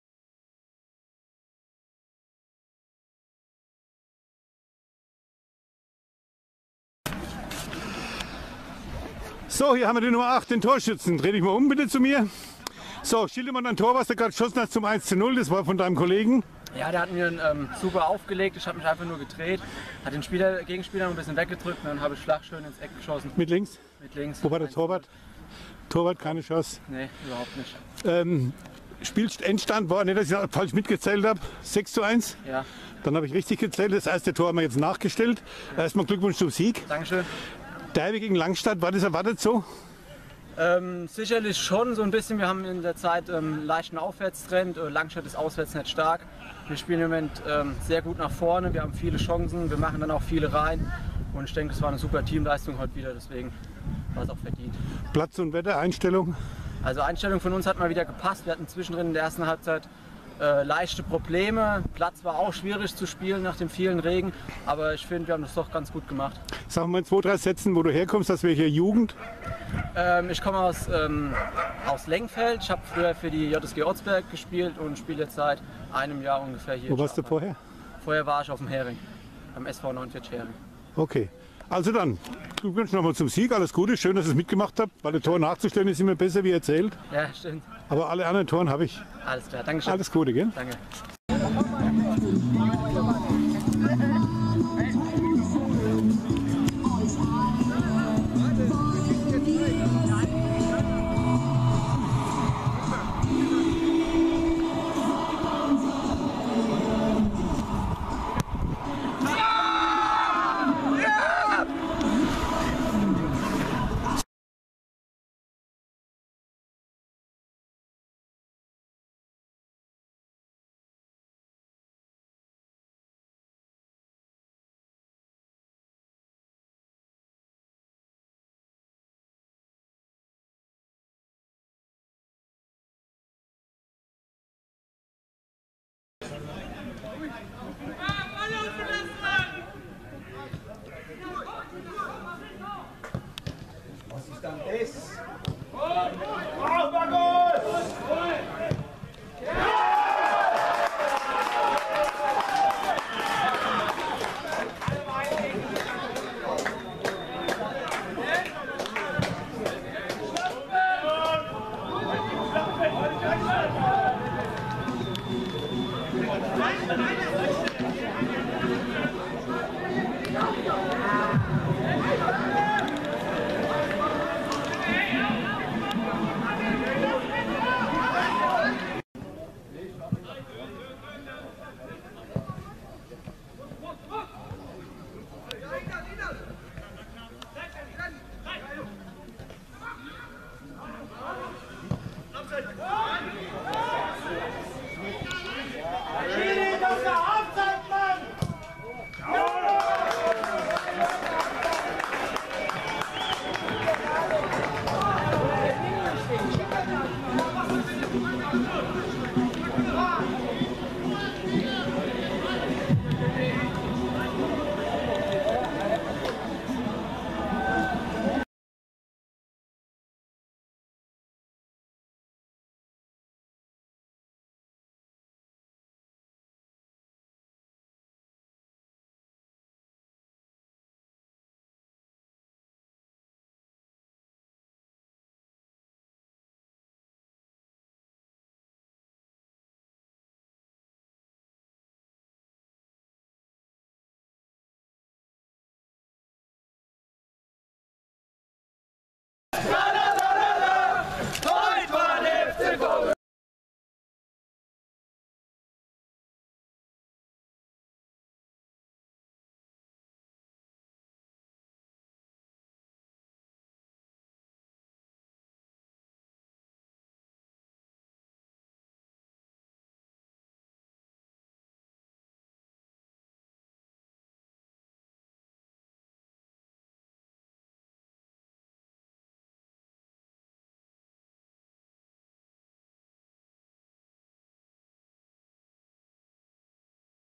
Schaafheim Langstadt Markus Lackner *21.8.1991–Sonntag 5.11.2017
14. Spieltag – Kreisoberliga Dieburg-Odenwald – Saison 2017-18 - Sonntag, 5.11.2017 – 14:30 Uhr - Zuschauer: 100
FC Viktoria Schaafheim (7.)  :  TSV Langstadt (10.)  6:1 (1:1)
Interview: Nr. 8  Markus Lackner * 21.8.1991